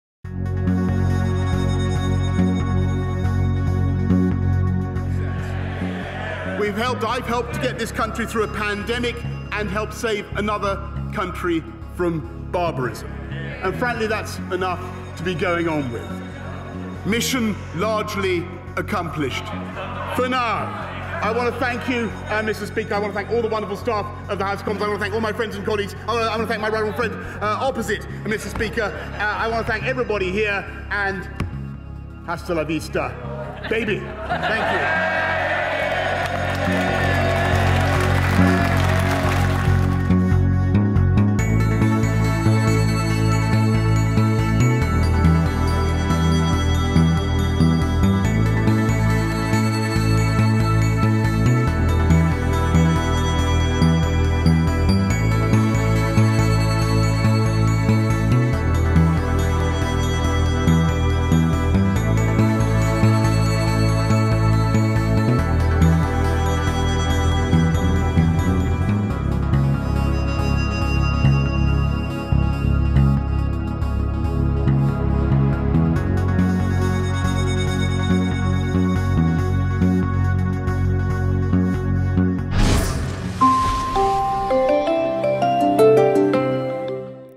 Johnson saying goodbye: Hasta la vista, baby
"Hasta la vista, baby!"Outgoing Prime Minister Boris Johnson decided to quote the terminator in his last speech at the PMQs dispatch box as the leader of the Conservative Party, and rightfully so, as his term almost saw him driving Britain to the ground.